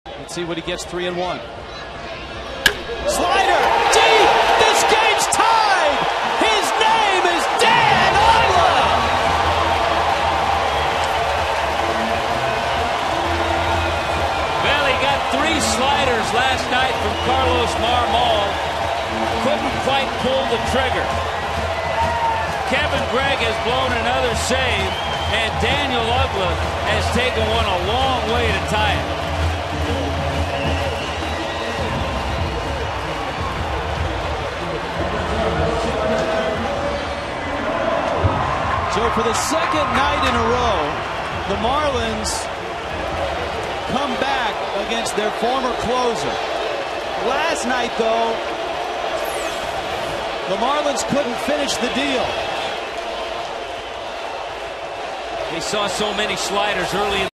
2009/08/02 Uggla's game-tying blast
CHC@FLA: Uggla ties the game with a solo homer